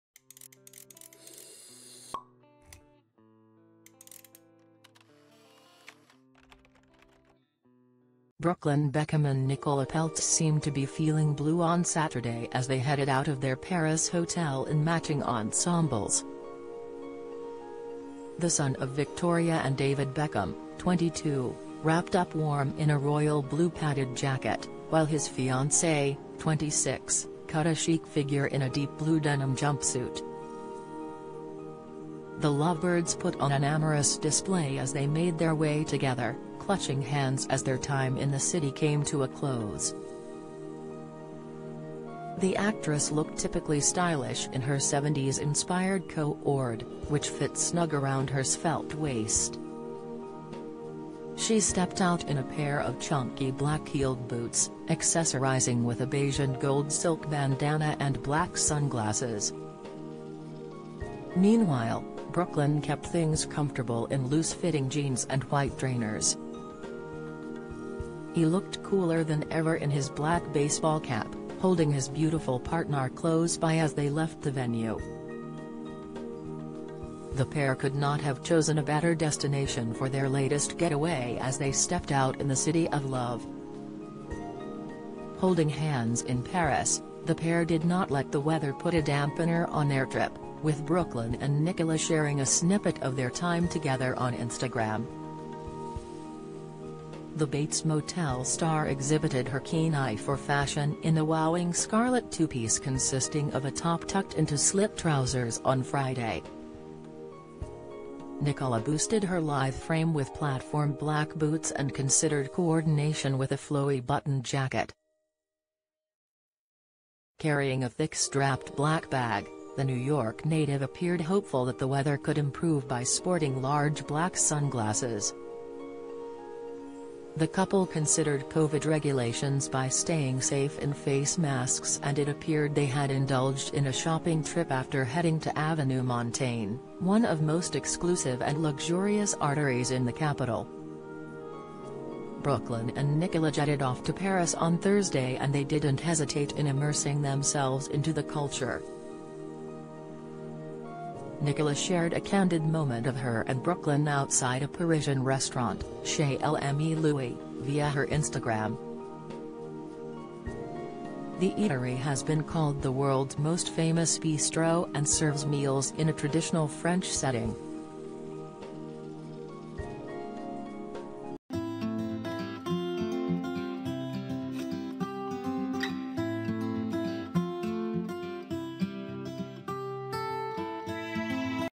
Brooklyn Beckham and Nicola Peltz put on an amorous display as they head out of their Paris hotel 05
Brooklyn Beckham and Nicola Peltz put on an amorous display as they head out of their Paris hotel 052233